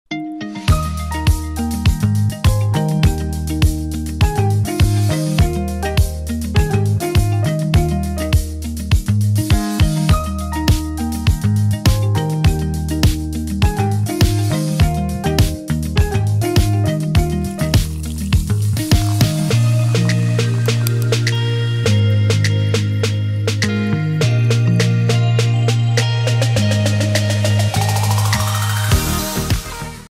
Cranberry Pomegranate Lime Spritzer
Simple and delicious! Make this cranberry pomegranate lime spritzer for your next get-together to really impress the group. Find the full recipe on Metro.ca!